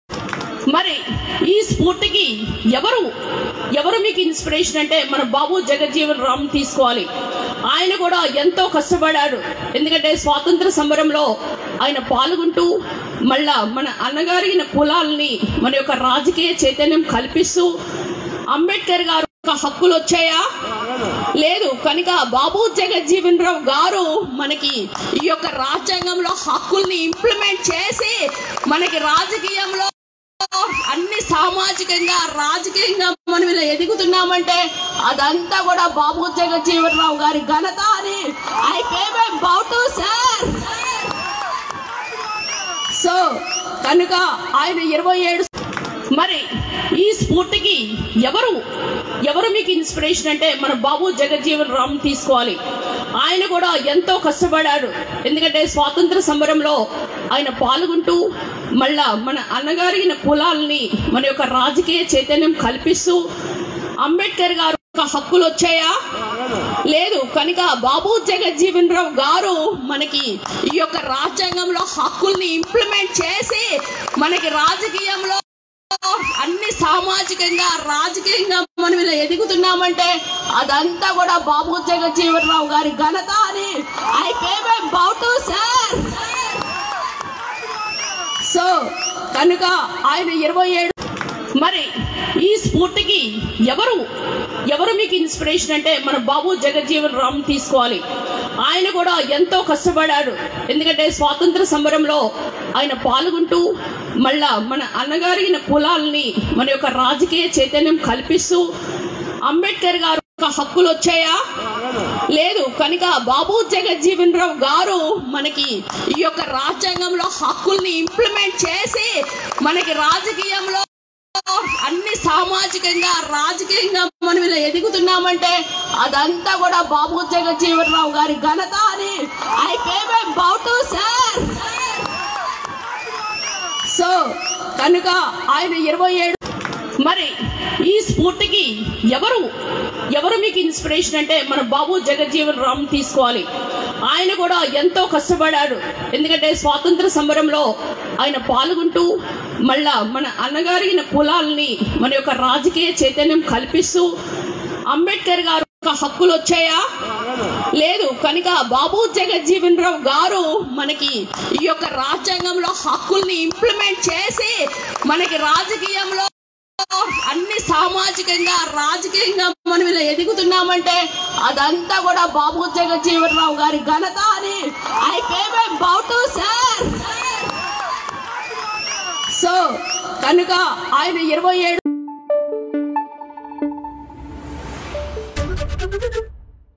YCP MLA Sridevi: డా.బీఆర్ అంబేడ్కర్ పై ఎమ్మెల్యే ఉండవల్లి శ్రీదేవి షాకింగ్ కామెంట్స్..! | ABP Desam
అంబేడ్కర్ పై తాడికొండ వైసీపీ ఎమ్మెల్యే ఉండవల్లి శ్రీదేవి వివాదాస్పద వ్యాఖ్యలు చేశారు. రాజ్యాంగ నిర్మాత అంబేడ్కర్ ను ఉద్దేశించి శ్రీదేవి చేసిన వ్యాఖ్యలు రాజకీయంగా పెద్ద దుమారం రేపుతున్నాయి.  ప్రపంచ మాదిగ దినోత్సవ ప్లీనరీలో పాల్గొన్న ఆమె మాట్లాడుతూ అంబేడ్కర్ వల్ల మాదిగలకు వచ్చిందేమీ లేదన్నారు. అంబేడ్కర్ వల్ల వచ్చిన హక్కులు ఏమీ లేవని శ్రీదేవి వ్యాఖ్యానించారు. అంబేడ్కర్ రాజ్యాంగాన్ని రాస్తే దానిని సక్రమంగా అమలు చేసిన బాబూ జగ్జీవన్ రామ్ వల్లే మాదిగలకు రాజ్యాంగ హక్కులు సంక్రమించాయని అన్నారు. ప్రతి ఒక్కరూ బాబూ జగ్జీవన్ రామ్ ను ఆదర్శంగా తీసుకోవాలని ఆమె పిలుపునిచ్చారు. మరోవైపు శ్రీదేవి వ్యాఖ్యలపై అంబేడ్కర్ అభిమానులు, ప్రజాసంఘాలు ఆగ్రహం వ్యక్తం చేస్తున్నాయి. ఎమ్మెల్యే శ్రీదేవి వెంటనే క్షమాపణలు చెప్పాలని డిమాండ్ చేస్తున్నారు.